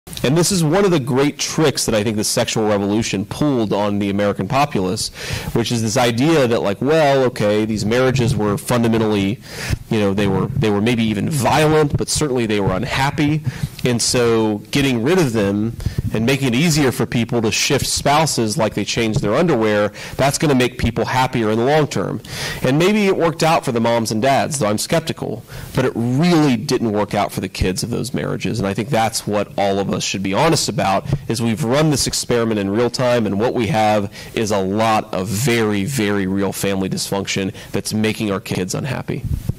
JD Vance says people in ‘Violent marriages' shouldn’t get divorced
Reported first by VICE: Ohio GOP Senate candidate J.D. Vance said last September that parents separating from "even violent" marriages made it "easier for people to shift spouses like they change their underwear" and that "didn't really work out for the kids of those marriages."